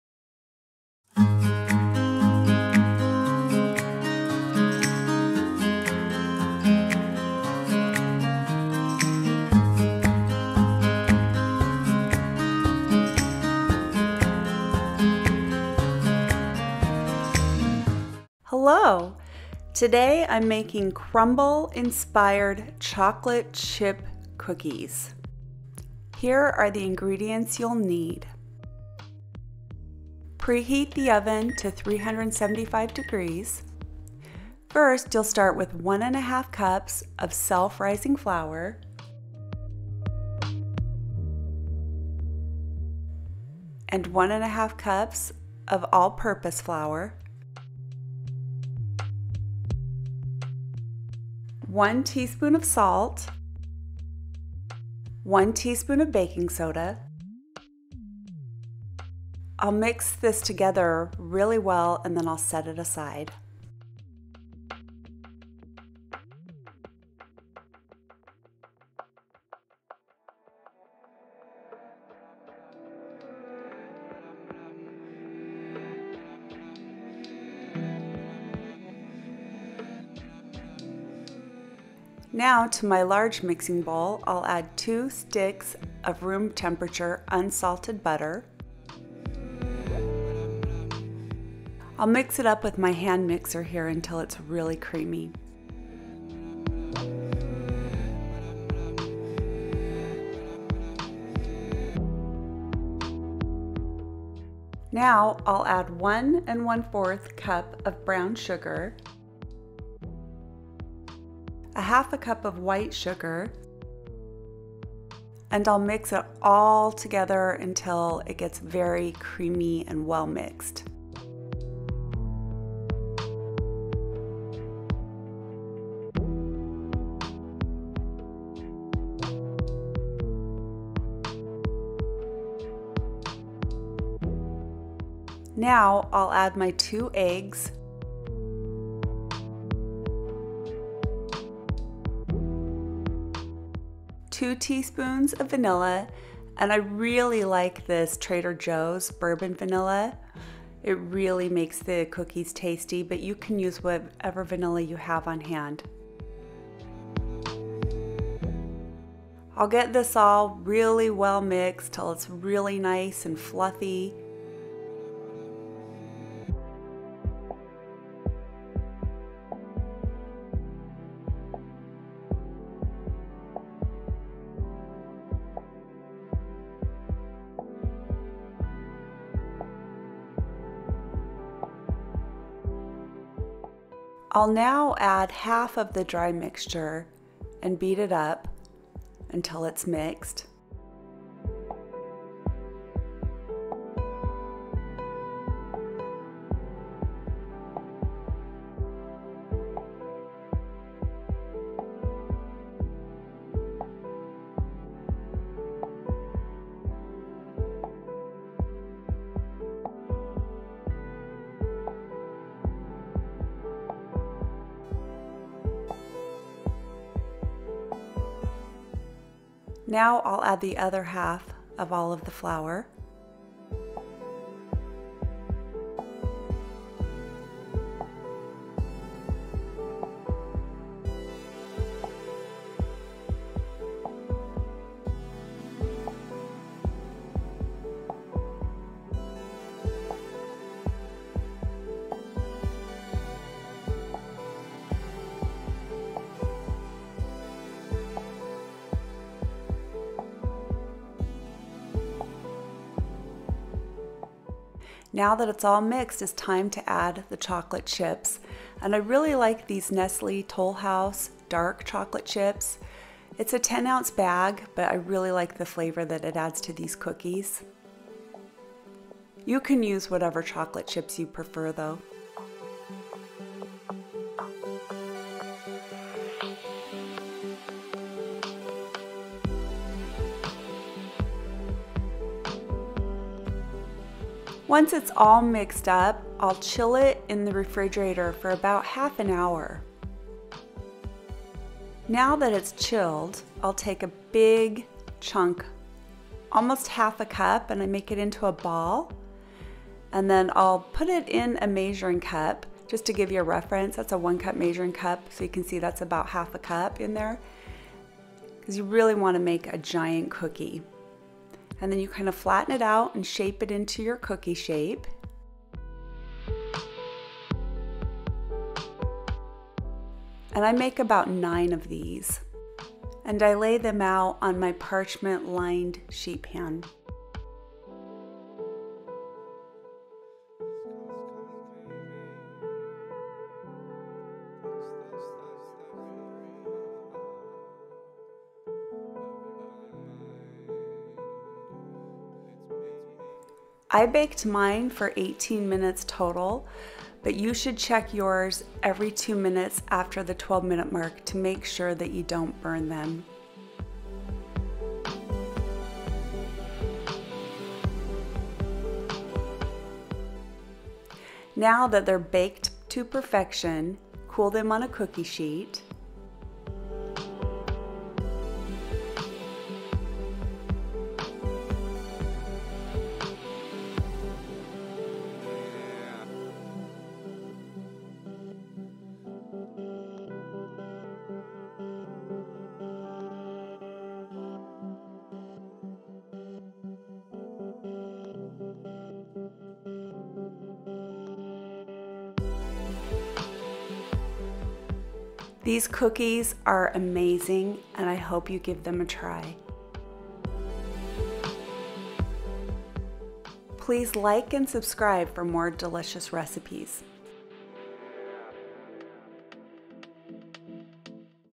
Crumbl Inspired Chocolate Chip cookies
These gigantic cookies hit the spot when I'm craving a Crumbl cookie.

Ingredients:
1 1/2 cups self-rising flour
1 1/2 cups all purpose flour
1 teaspoon salt
1 teaspoon baking soda
2 cubes unsalted butter (room temperature)
1 1/4 cup brown sugar
1/2 cup white sugar
2 eggs
2 teaspoons vanilla
10 oz Nestle dark chocolate chips

Follow video for step by step directions